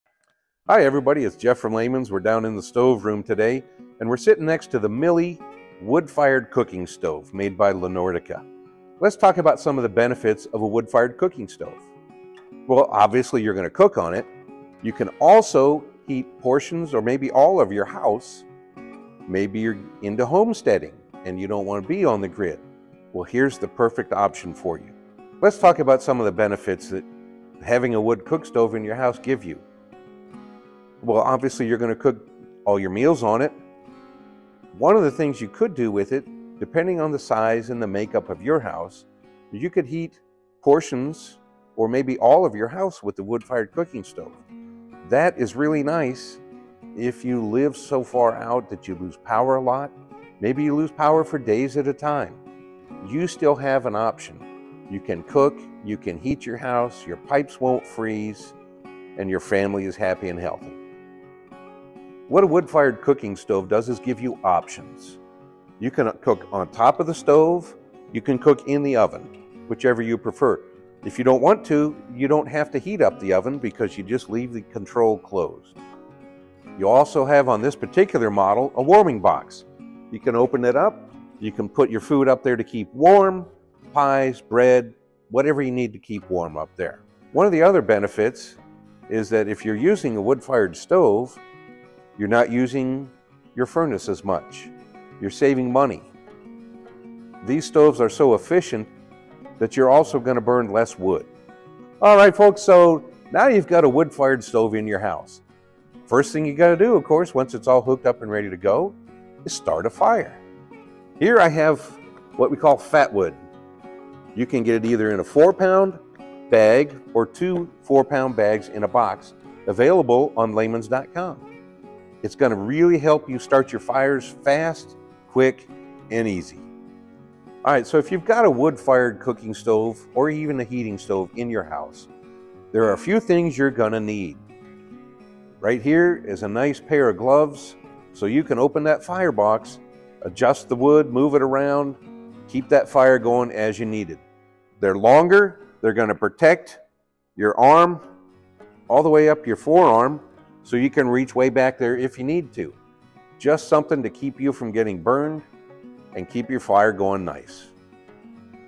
La Nordica Milly Wood Cook Stove
As the weather becomes cooler, look for options to heat your home. Join Jeff as he talks about the La Nordica Milly Wood Cook Stove and the benefits to having one on your homestead.